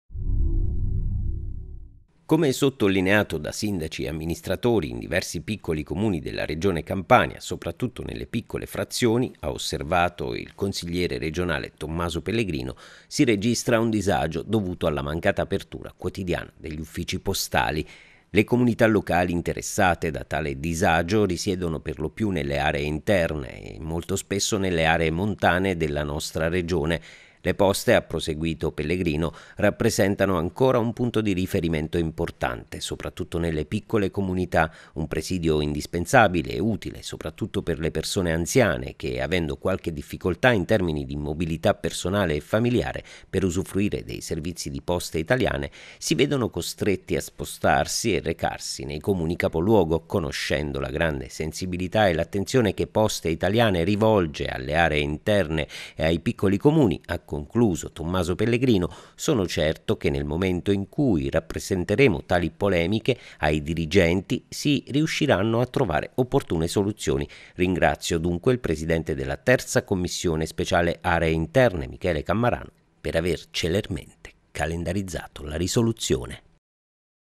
Campania, Uffici postali, Pellegrino “Garantire presidi e aperture quotidiane nelle aree interne"
“Come sottolineato da  Sindaci e Amministratori, in diversi piccoli Comuni della Regione Campania, soprattutto nelle piccole frazioni  – ha osservato Pellegrino - si registra un disagio dovuto alla mancata apertura quotidiana degli uffici postali. Le Comunità locali interessate da tale disagio risiedono per lo più nelle aree interne e molto spesso nelle aree montane della nostra Regione.